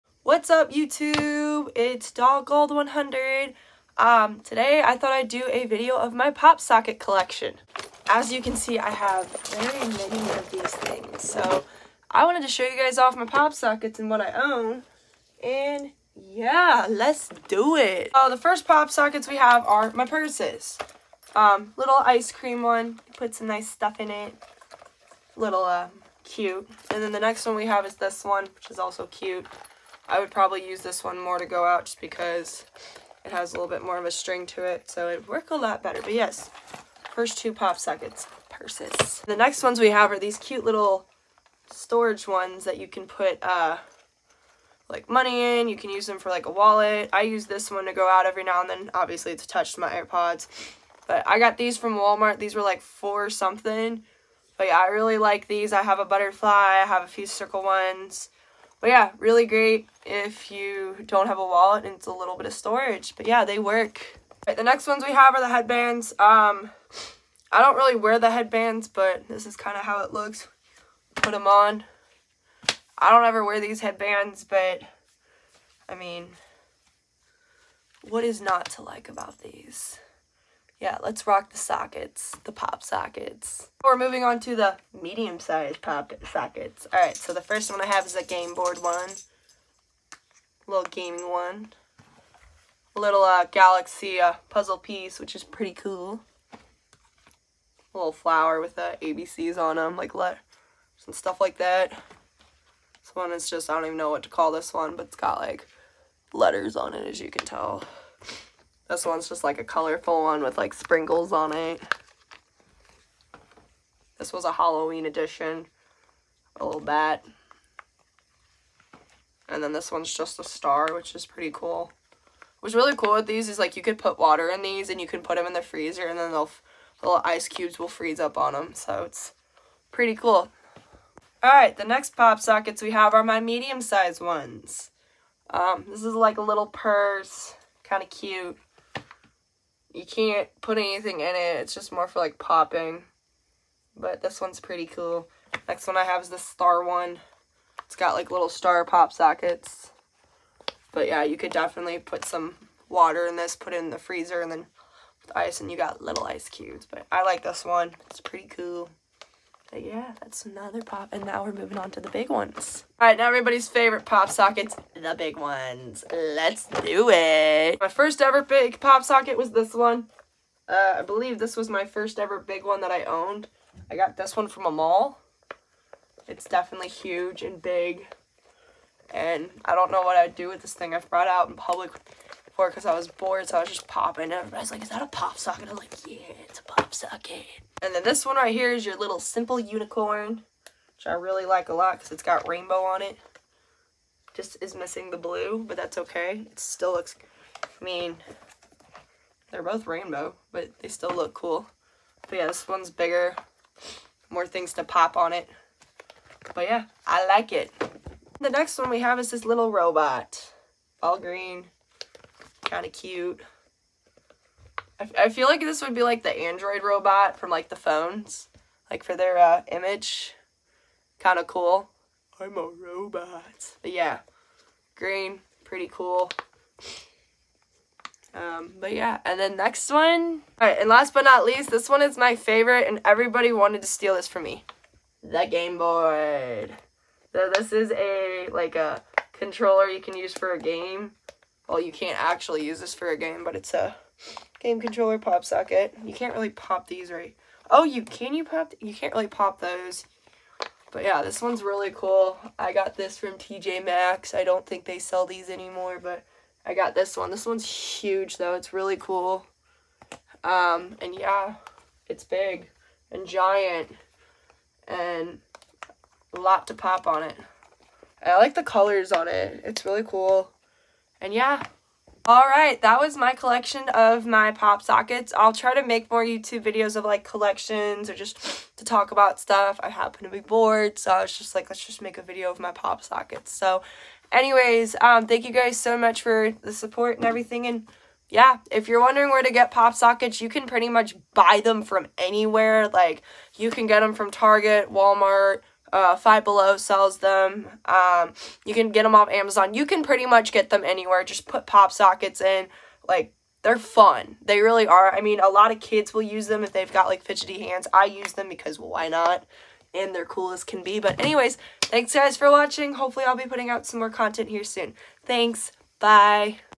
My pop socket collection￼
Just showing off my PopSocket collections these things are really fun to collect and pop around.

My Social:
Tik Tock: @Modelclaudiamary